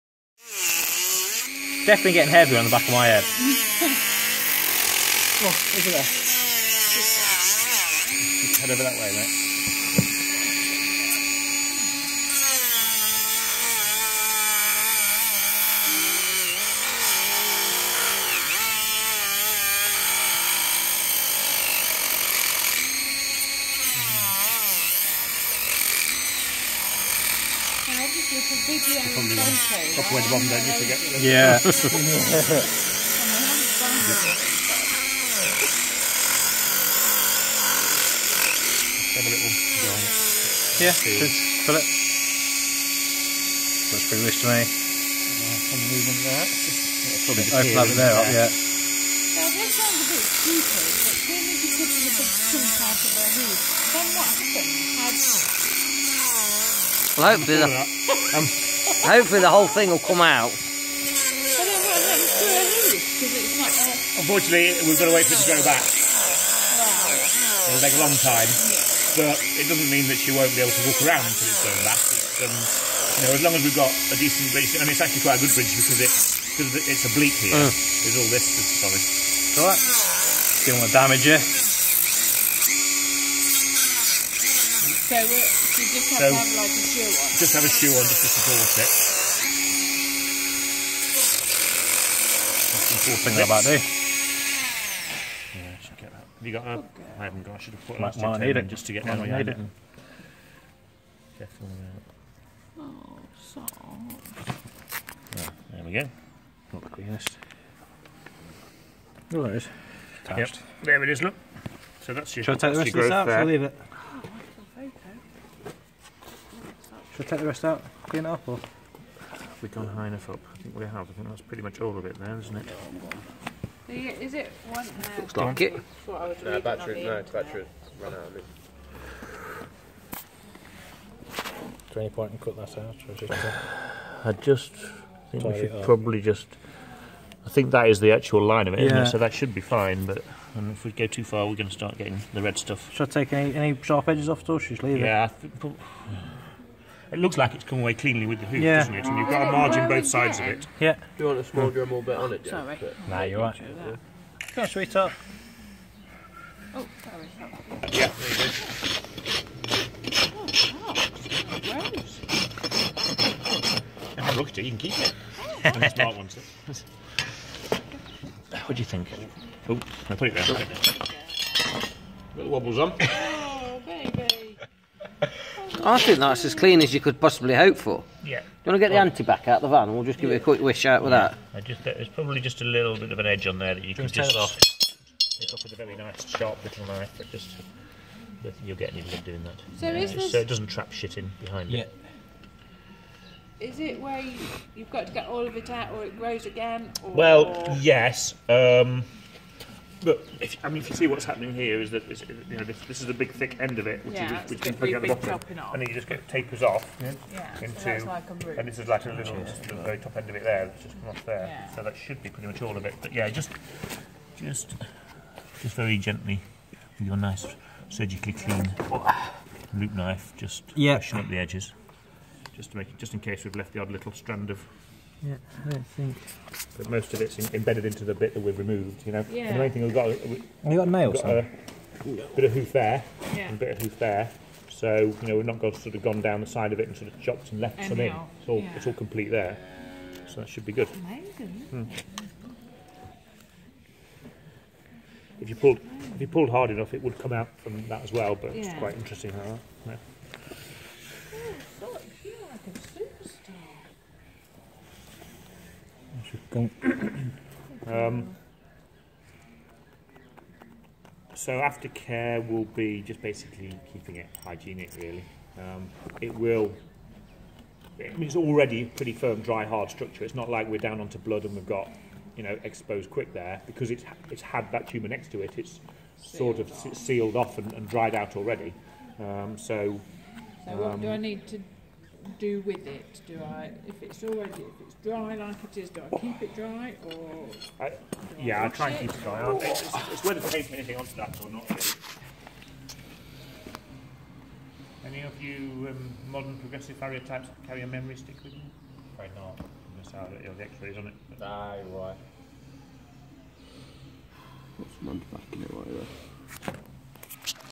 Helping Sox: shoeing a horse with a Keratoma
Watch as a farrier removes a disturbing cylindrical keratoma from inside a horse's hoof. This rare condition can cause severe discomfort for the animal.